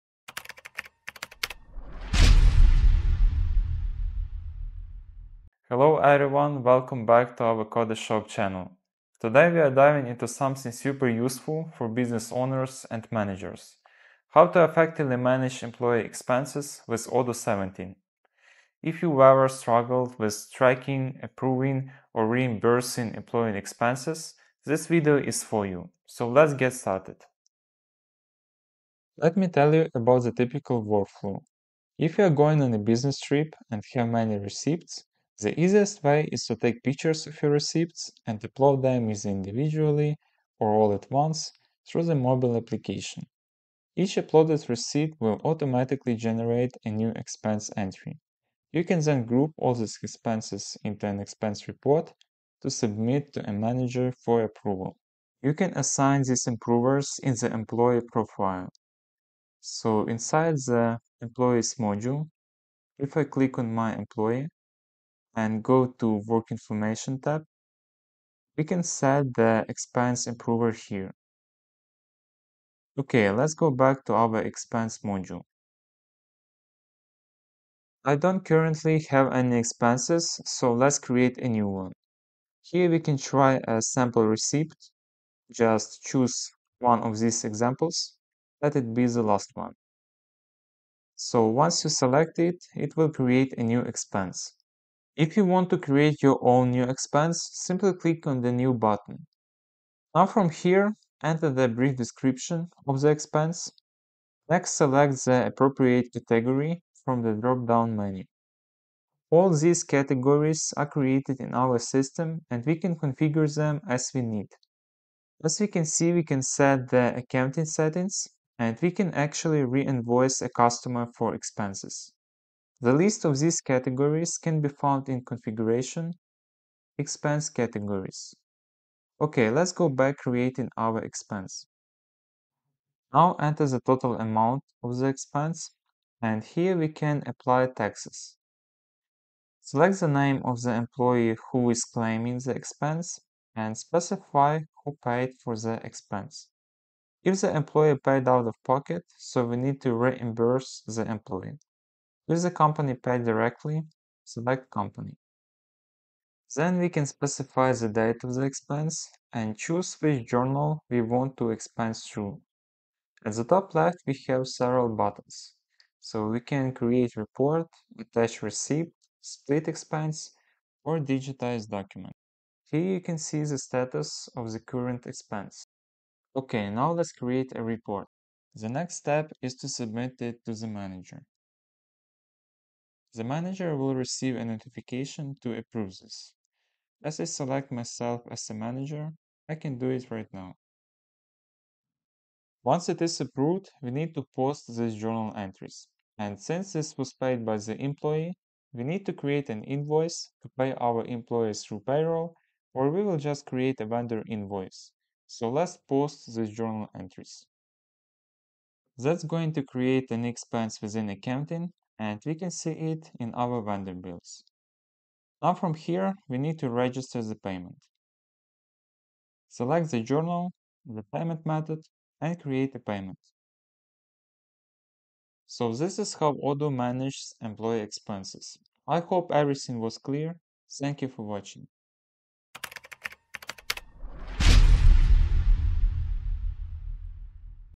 How to Manage Employee Expenses Using Odoo 17
In this video, we explore how to efficiently manage employee expenses using Odoo 17. Whether you're a business owner or manager, this guide will help you streamline the process of tracking, approving, and reimbursing expenses.

We'll walk you through the typical workflow: capturing receipts, creating expense entries, and compiling them into a report for approval. Learn how to assign expense approvers, configure expense categories, and handle reimbursements.

Watch now to master Odoo's expense management features and keep your financial processes smooth and efficient!